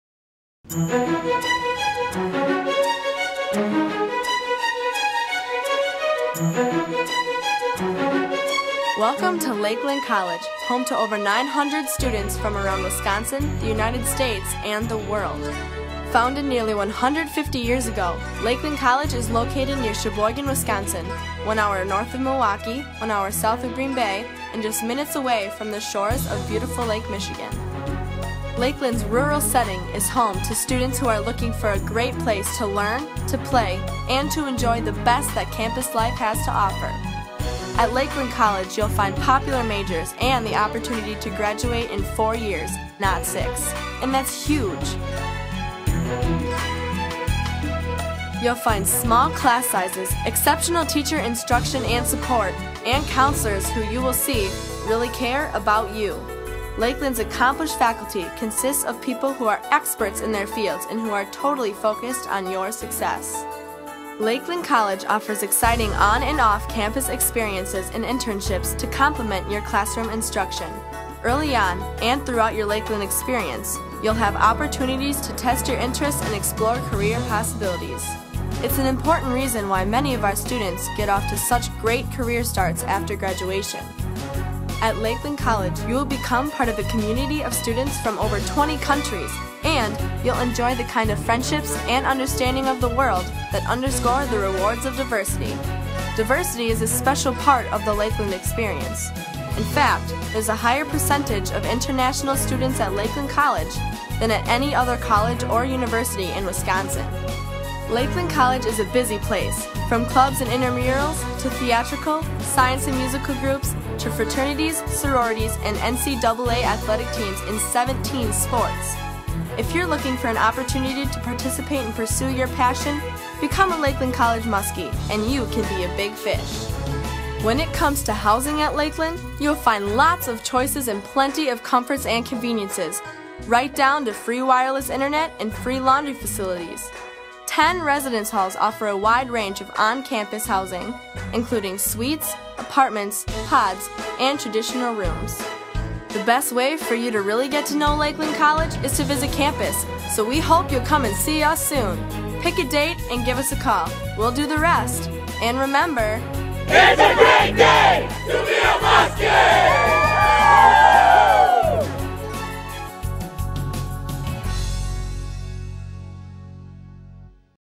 Lakeland College (WI)
An overview of Lakeland College, located just outside of Sheboygan, WI.